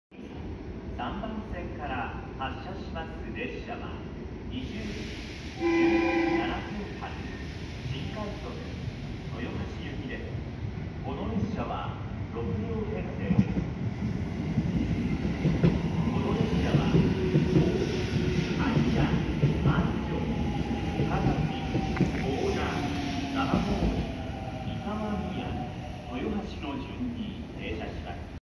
●20201224 313系 到着 ＠金山
撮影路線：JR東海 東海道本線
撮影駅：金山
行先：区間快速 名古屋
車両：313系
両数：4両
撮影機材：【スマホ】SONY Xperia XZ2 Premium
MH：ーー
備考欄：